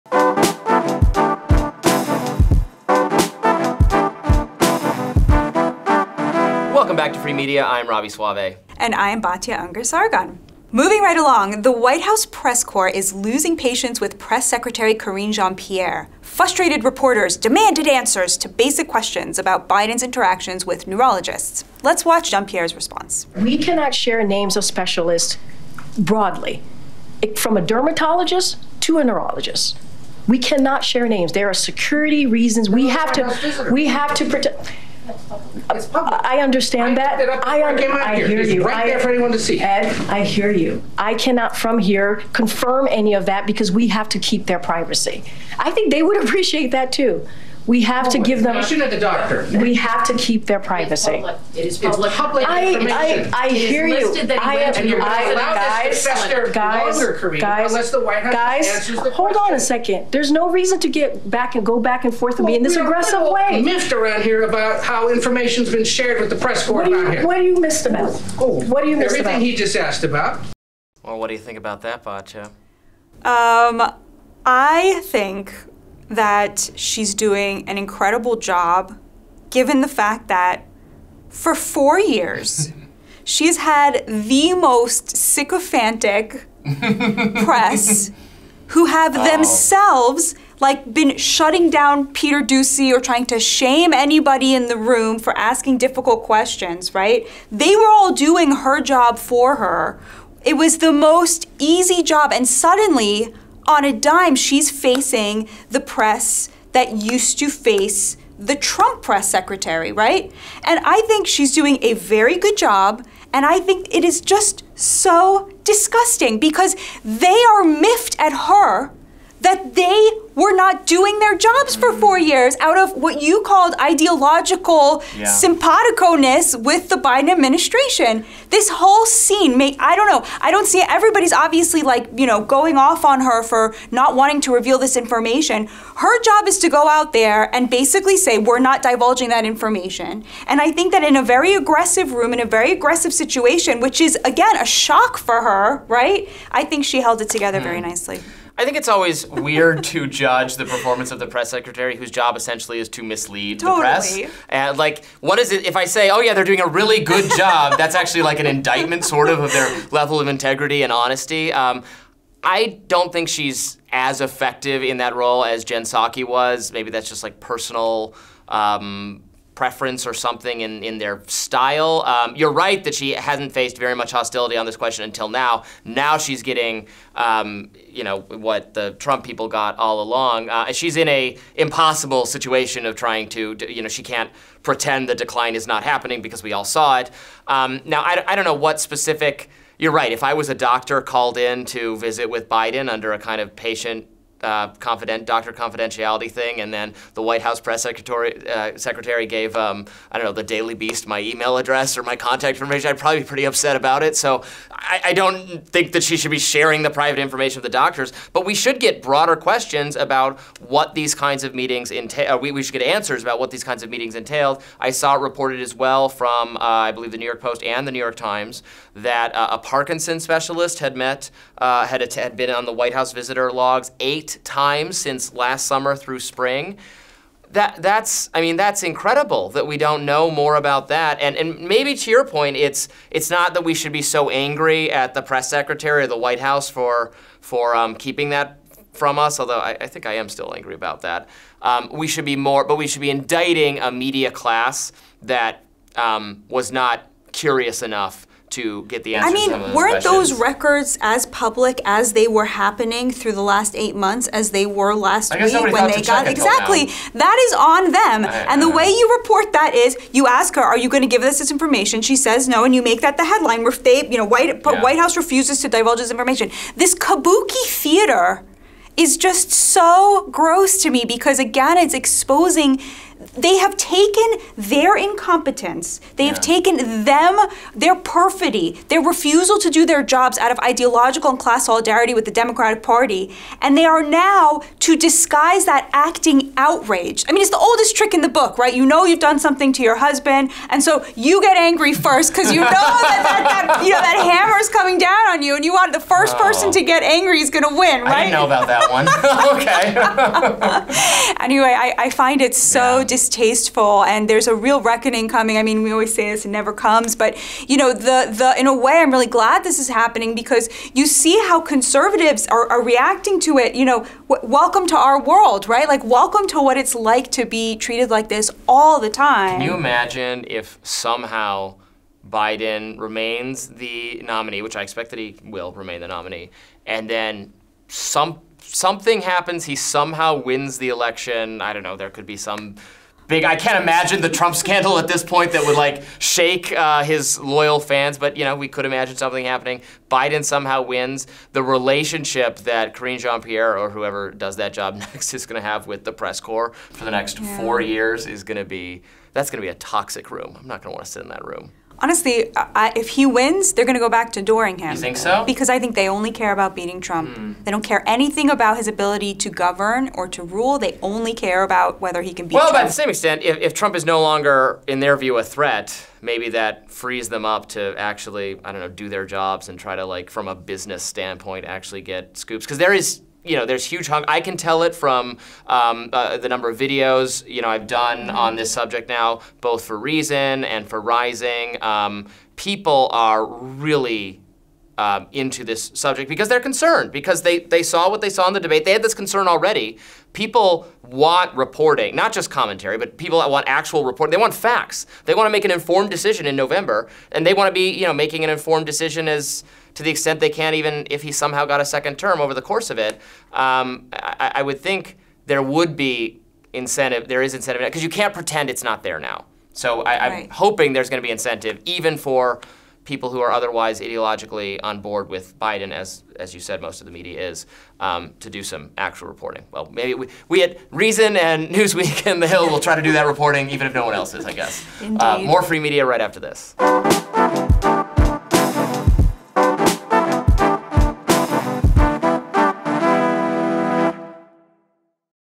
Karine Jean-Pierre REFUSES to answer questions about Biden’s neurologist
Robby Soave and Batya Ungar-Sargon discuss how Press Secretary Karine Jean-Pierre is fighting off the press who refused to report on Biden's mental acuity until now.

Producer: Natalie Dowzicky
Director of Photography: Alex Rosen
Photo Credit: MSNBC